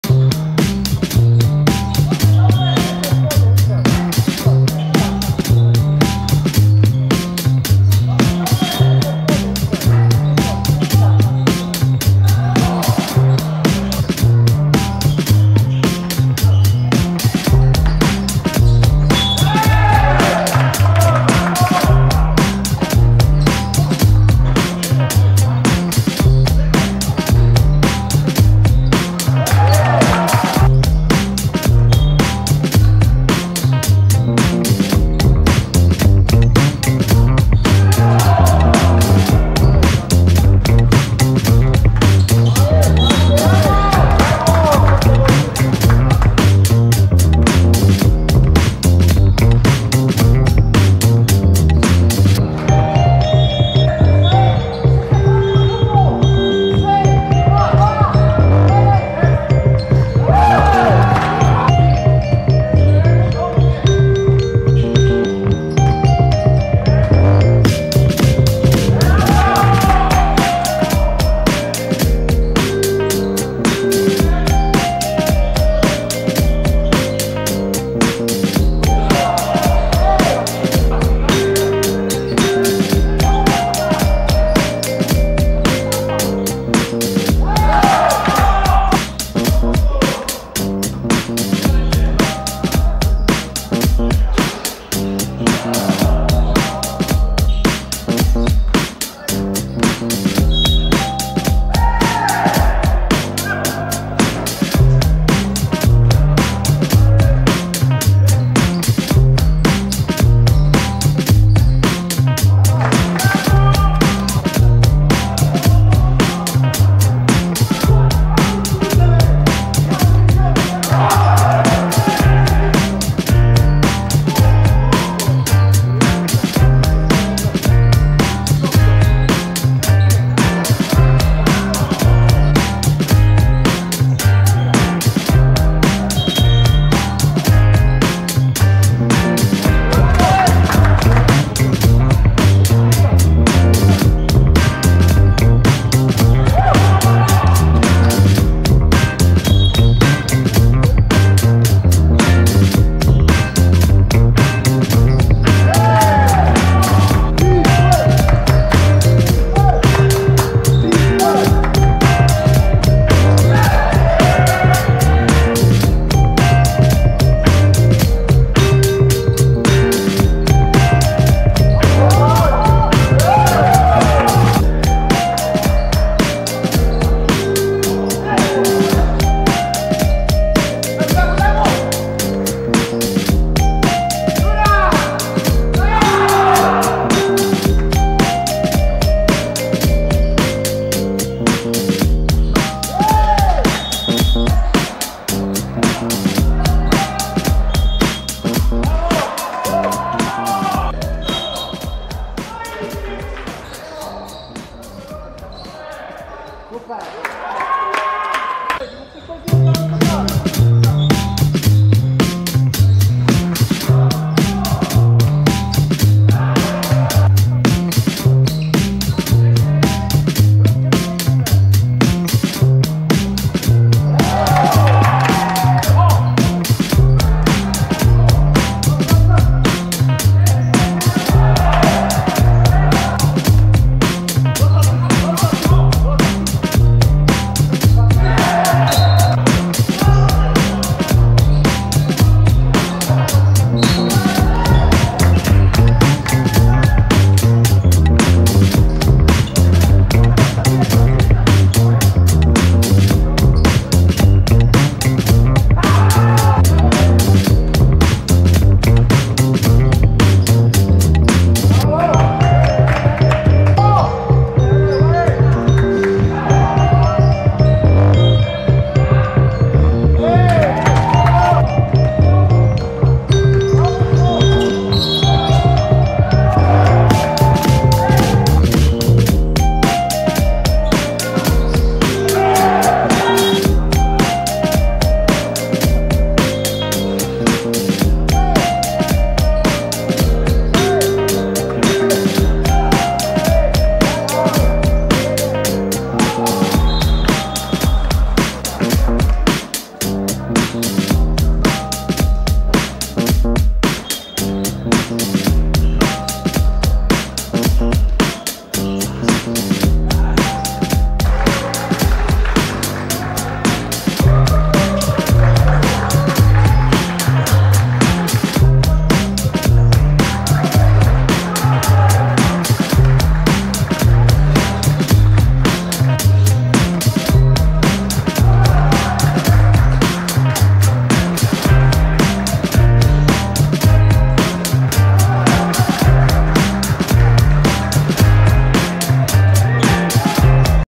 09 05 Finał Chicago vs New Jersey – Ursynów Wiosna 2022
CHICAGO 56:66 NEW JERSEY
M.Kosierb 27pkt 4x3 9-9ft 13zb
A.Pasternak 16pkt 5zb 5ast